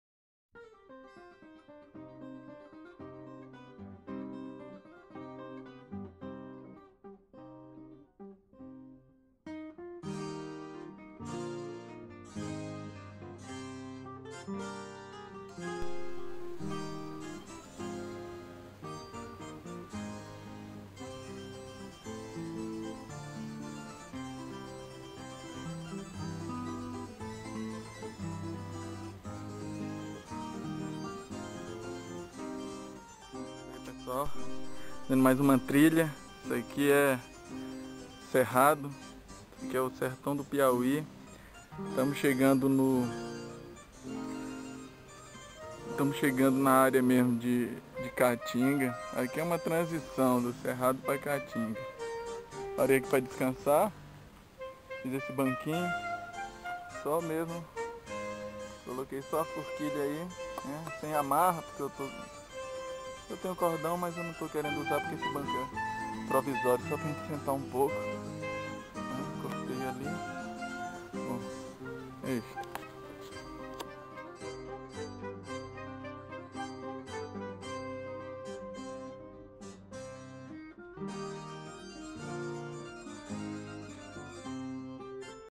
Reconhecimento de área para acampamento e bushcraft no sul do Piauí
Este vídeo foi graqvado em abril de 2011 e mostra um pouco do cerrado do sul do Piauí em uma transição para a caatinga.